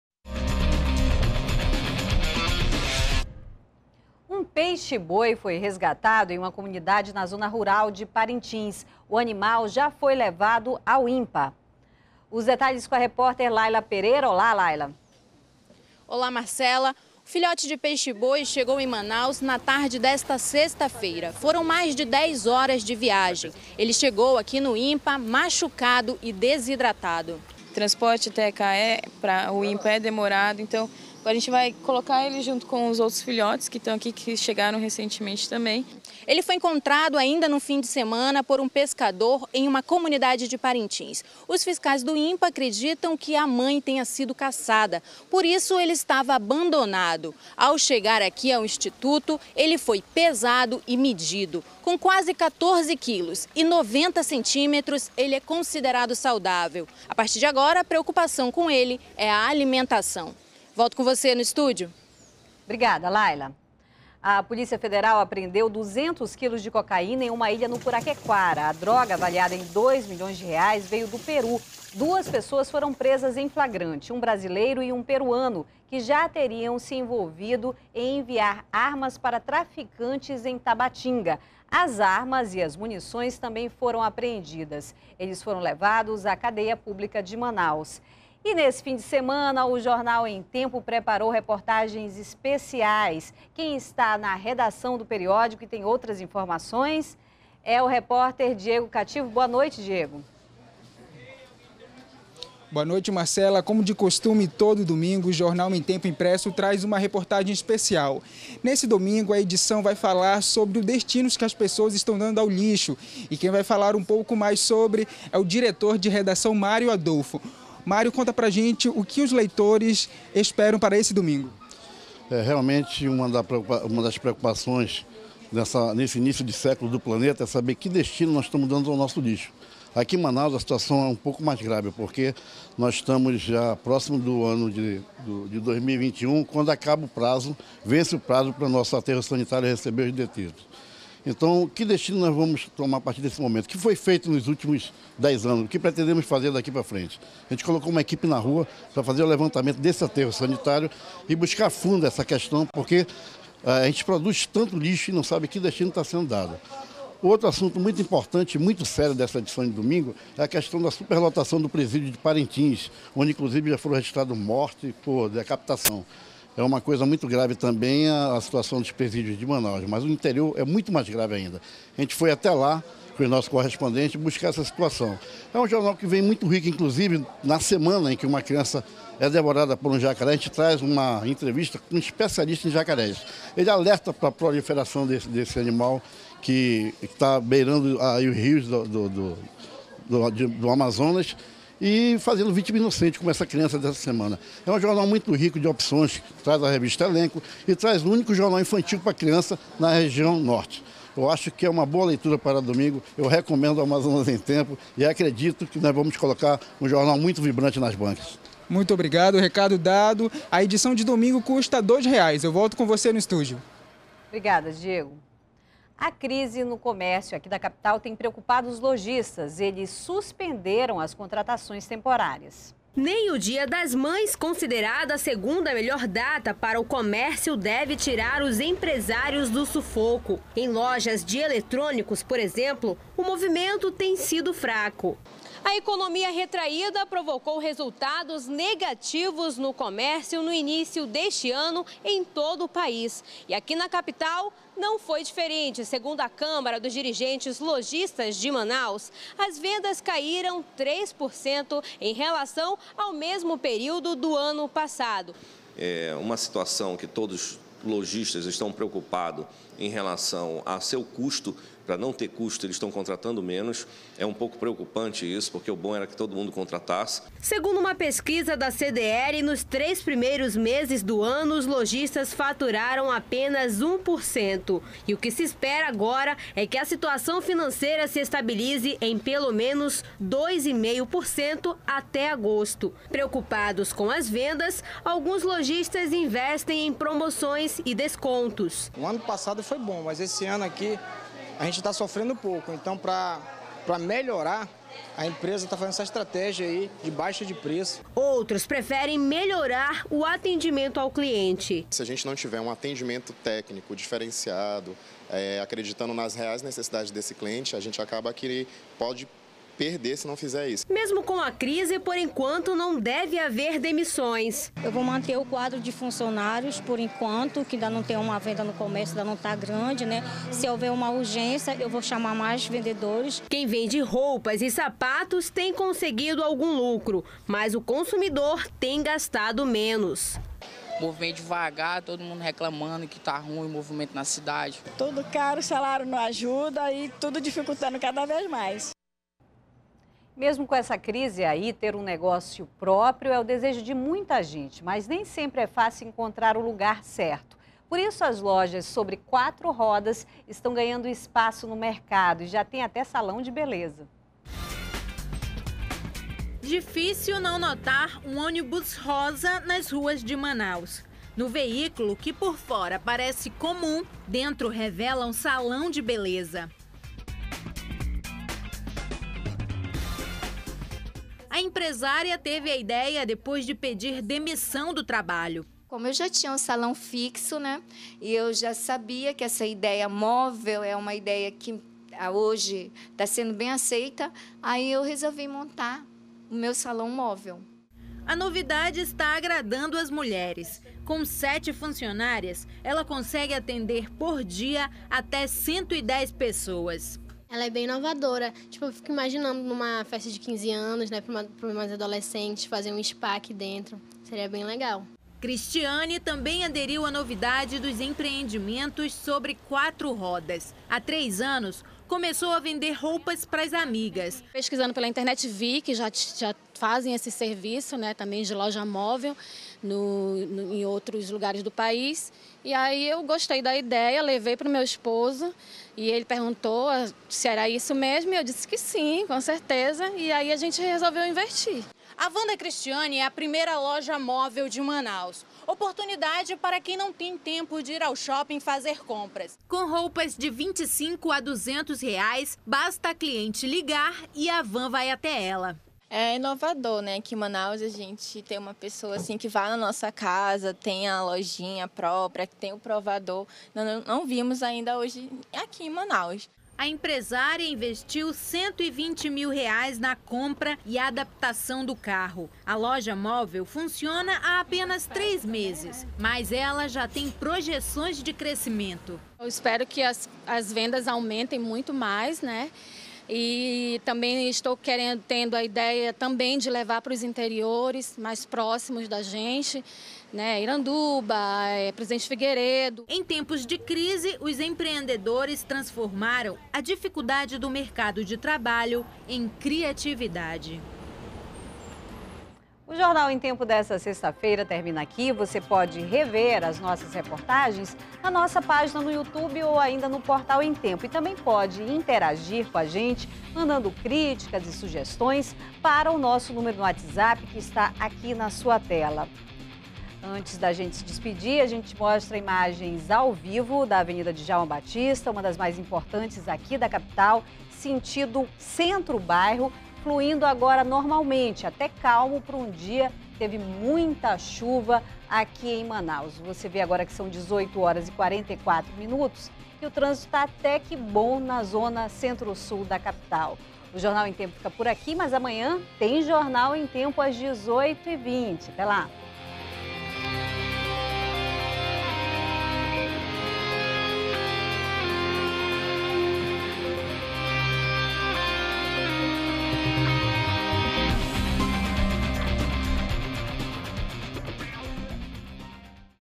Peixe-boi é resgatado em uma comunidade na zona rural de Parintins - AM
03º BLOCO JORNAL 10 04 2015 - Peixe-boi é resgatado em uma comunidade na zona rural de Parintins - AM